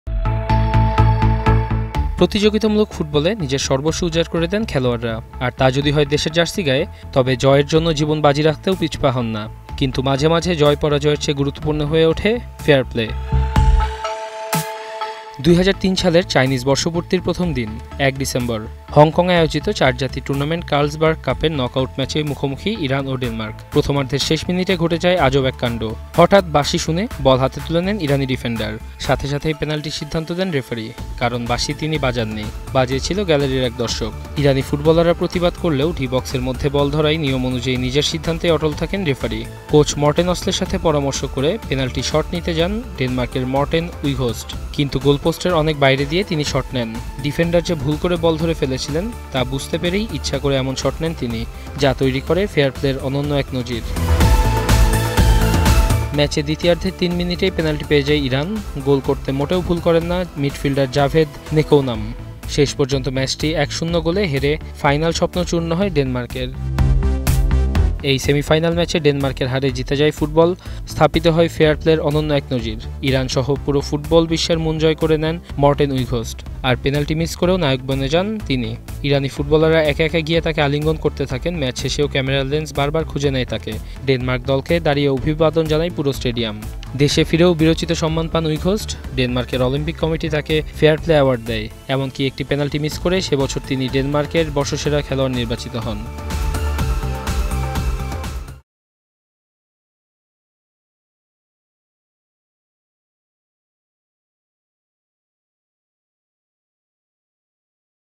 ম্যাচ হেরেও ফুটবলবিশ্বের মন জয় করার গল্প | Denmark Vs Iran | Morten Wieghorst | Ekhon TV
প্রতিযোগিতামূলক ফুটবলে নিজের সর্বস্ব উজাড় করে দেন খেলোয়াড়রা। আর তা যদি হয় দেশের জার্সি গায়ে; তবে জয়ের জন্য জীবন বাজি রেখতেও পিছুপা হন না। কিন্তু মাঝে মাঝে জয় পরাজয়ের চেয়ে গুরুত্বপূর্ণ হয়ে উঠে ফেয়ার প্লে।

Our Office Address:
EKHON TV
City Park Lane
19 Hatkhola Road, Wari
Dhaka-1203
Bangladesh